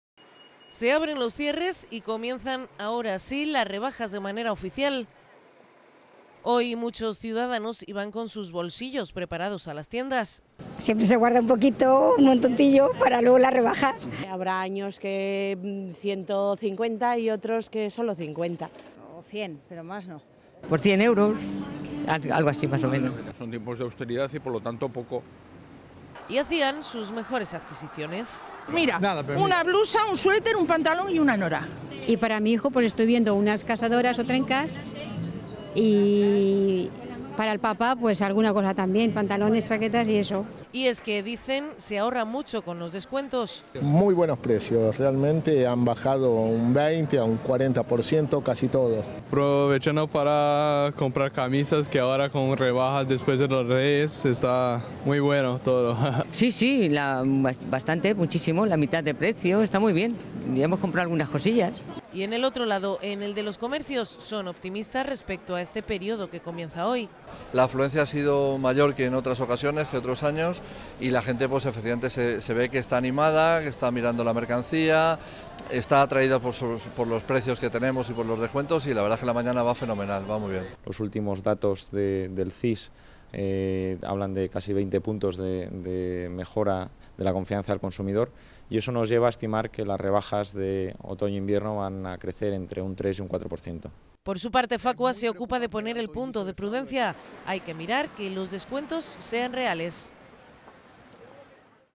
Las rebajas animan las compras tras los Reyes
Se abren los cierres y comienzan, ahora sí, las rebajas de manera oficial. Hoy muchos ciudadanos iban con sus bolsillos preparados a las tiendas y hacían sus mejores adquisiciones. Y en el otro lado, los comerciantes son optimistas respecto a este perídodo que comienza hoy.